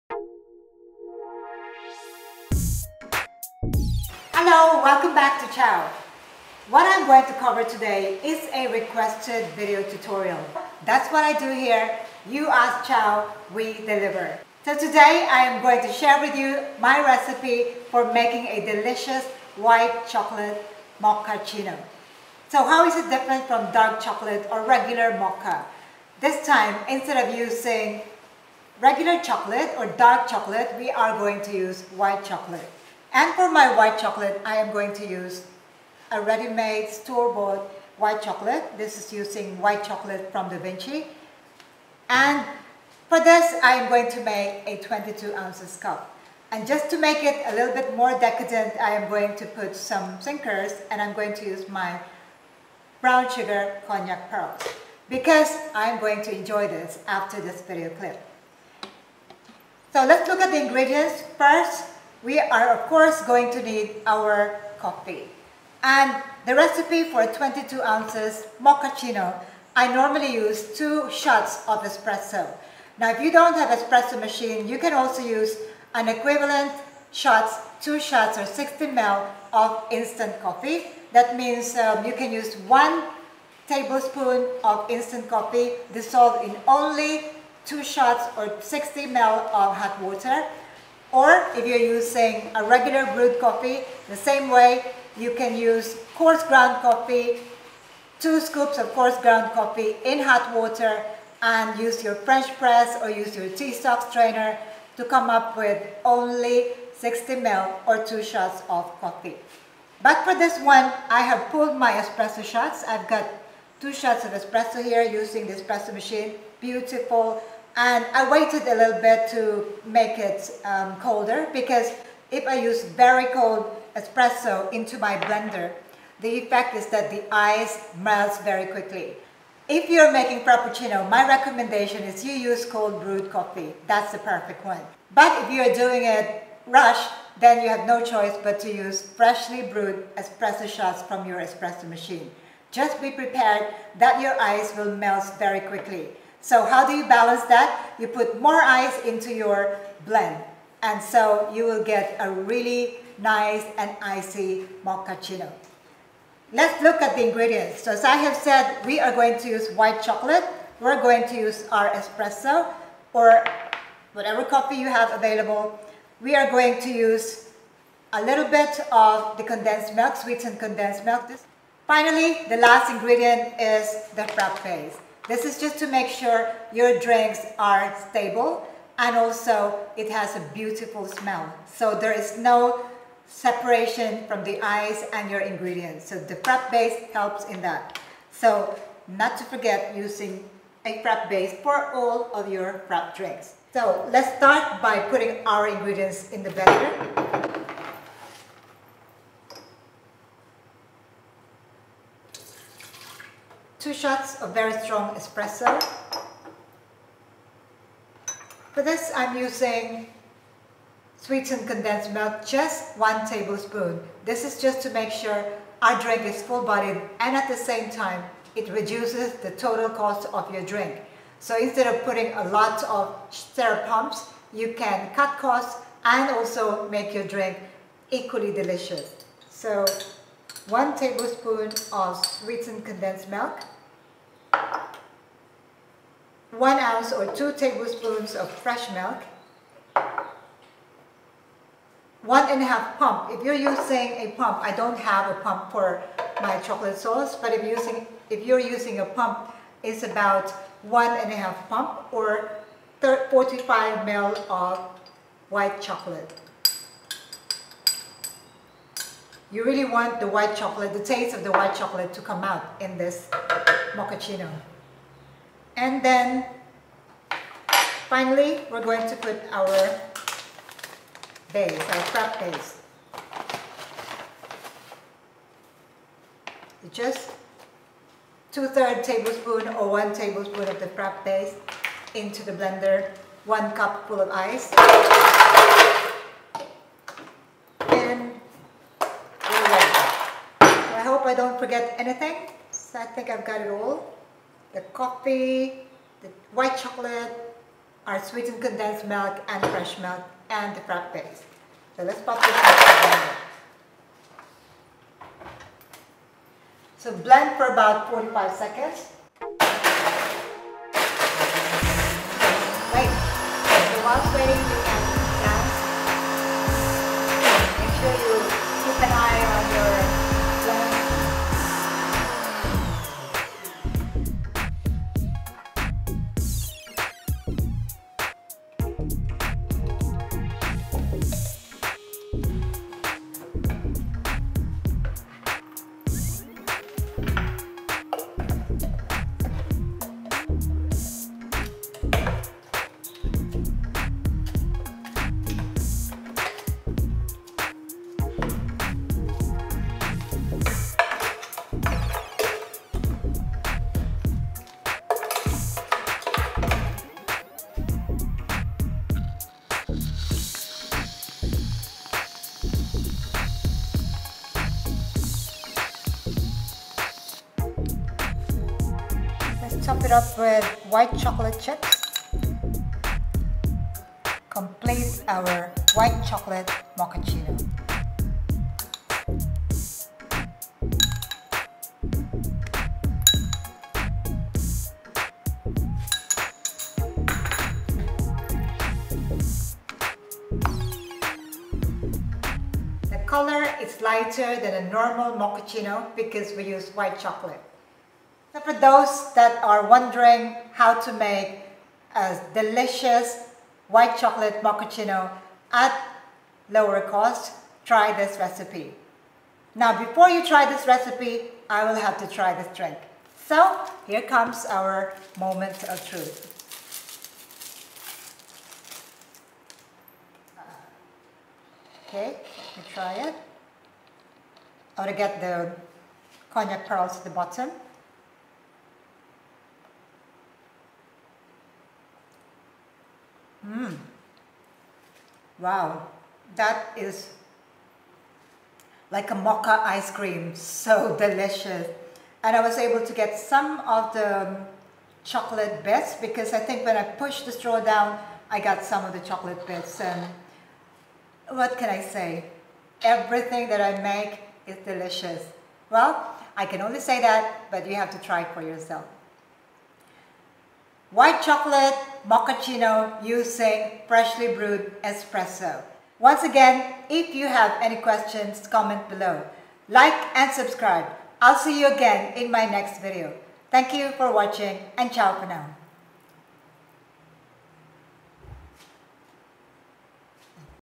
DELICIOUS RECIPE FOR WHITE CHOCOLATE MOCHACCINO
1oz = 30ml

RECIPE:22oz
2 Shots Espresso (or instant coffee or regular brewed coffee = 60ml)
45 ml White Chocolate Sauce
15 ml Sweetened Condensed Milk
30 ml Fresh Milk
1 tbsp Frappe Base
White Chocolate Chips topping with whipped cream
Ice
Sinkers (Optional). I used brown sugar Konjac Pearls here :-)

📚For milk tea and coffee shop supplies available at Chao, please